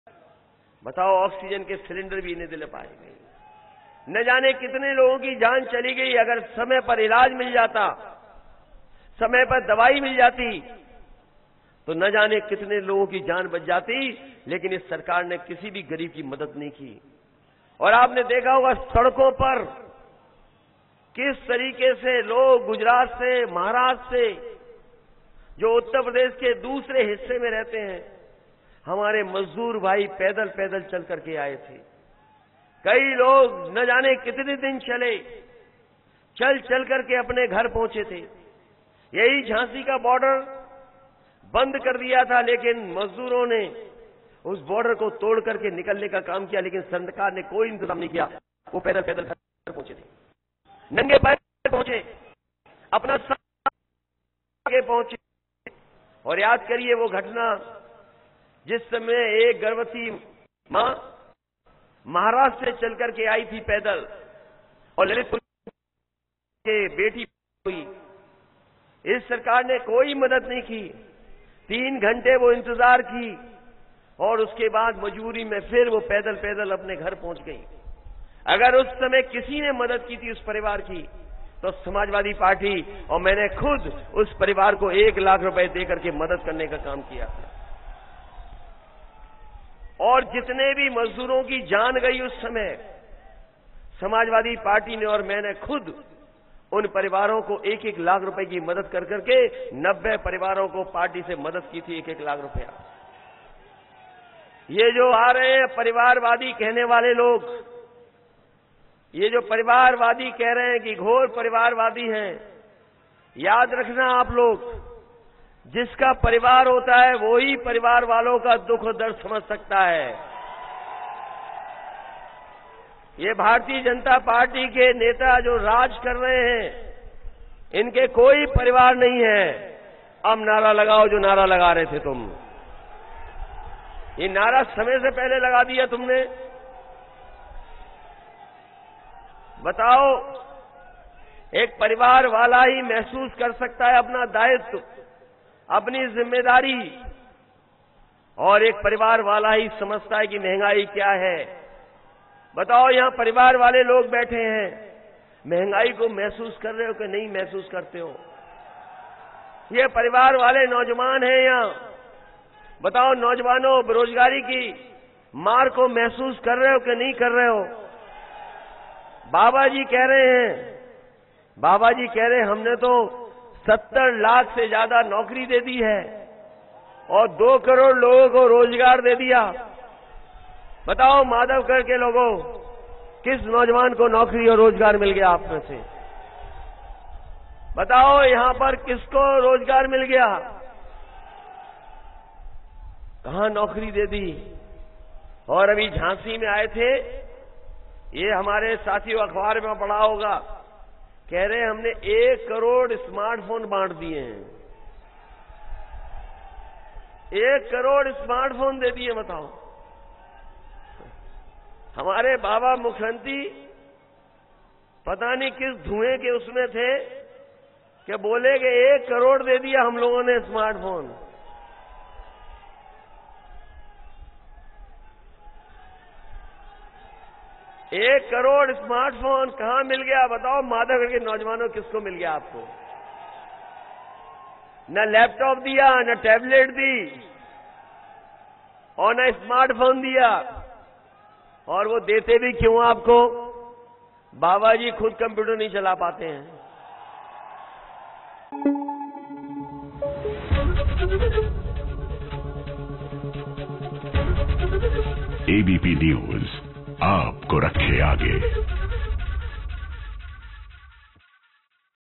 गरीबों का पैसा बड़े उद्योगपतियों को दे रही है बीजेपी सरकार- बोले Akhilesh Yadav
Akhilesh Yadav बोले- गरीबों का पैसा बड़े उद्योगपतियों को दे रही है बीजेपी सरकार..